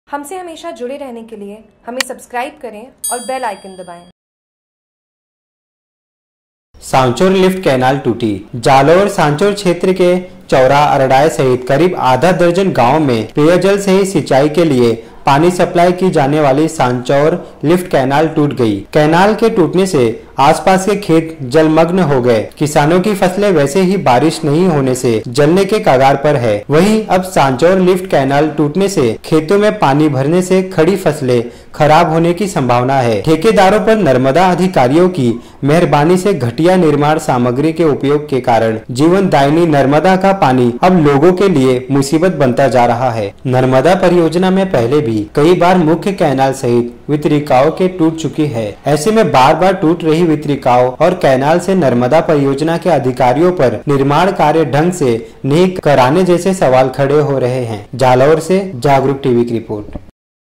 JAGRUK TV : जालोर : सांचौर लिफ्ट केनाल टूटी
जालोर।सांचौर क्षेत्र के चौरा,अरणाय सहित करीब आधा दर्जन गांवो मे पैयजल सहित सिचांई के लिए पानी सप्लाई की जाने वाली सांचौर लिफ्ट कैनाल टुट गई  ।कैनाल के टुटने से आसपास के खेत जलमग्न हो गये।किसानो की फसले वैसे ही बारिश नही होने से जलने के कगार पर है वही अब सांचौर लिफ्ट कैनाल टुटने से खेतो मे पानी भरने से खडी फसले खराब होने की संभावना है ठेकेदारो पर नर्मदा अधिकरियो की मेहरबानी से घटिया निर्माण सामग्री के उपयोग के कारण जीवनदायिनी नर्मदा का पानी अब लोगो के लिए मुसीबत बनता जा रहा है नर्मदा परियोजना मे पहले भी कई बार मुख्य कैनाल सहित वितरिकाओ के टुट चुकी है ऐैसे मे बार बार टुट रही वितरिकाओ व कैनाल से नर्मदा परियोजना के अधिकारियो पर निर्माण कार्य ढंग से नही कराने जैसे सवाल खडे हो रहा है